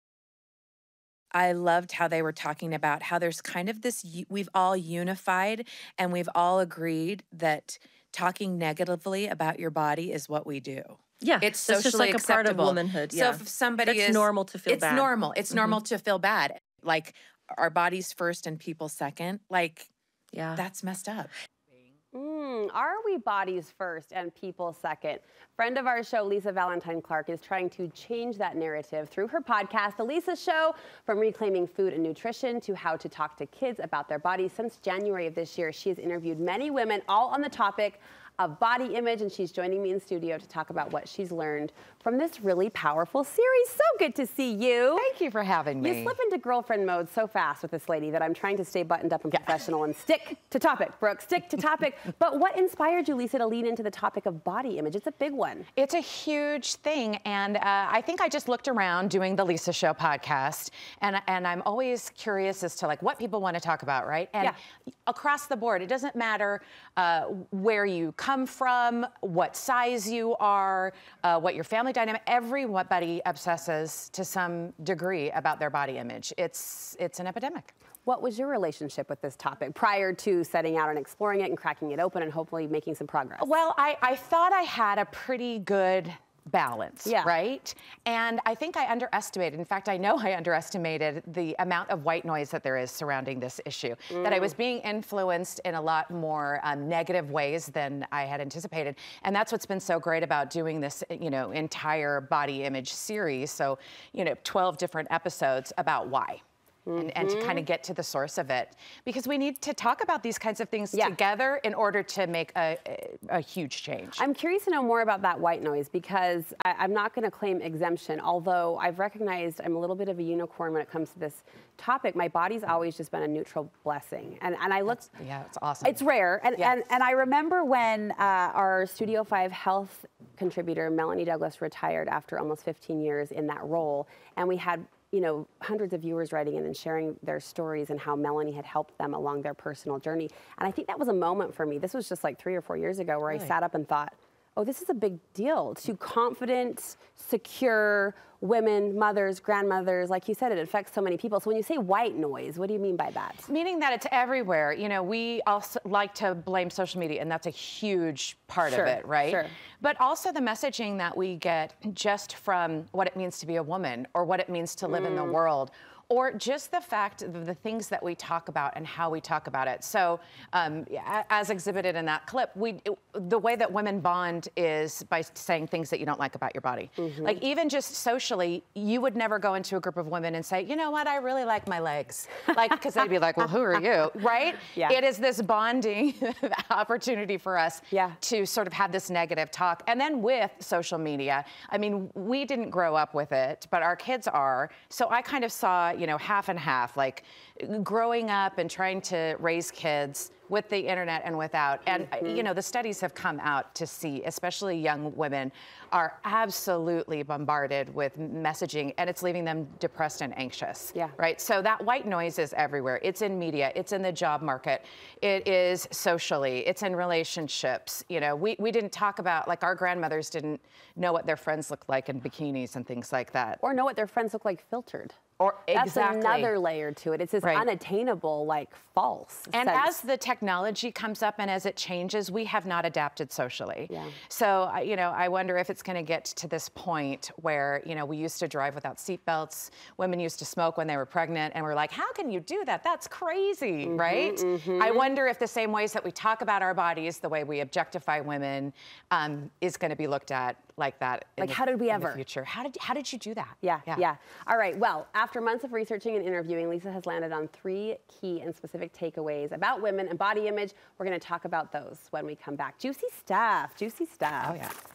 Women are not bodies first and people second. Lisa Valentine Clark talks body image
Friend of the show Lisa Valentine Clark is trying to change the narrative that women are bodies first and people second through her podcast "The Lisa Show. Since January of this year, she's interviewed many women - all on the topic of body image. The topics include reclaiming food and nutrition, how to talk to kids about their bodies, finding the right reason to exercise, and more.

Lisa shares three personal takeaways from the series.

You can listen to “The Lisa Show” on any podcast platform. You can also find more from Lisa on Instagram @yourfunnyvalentine.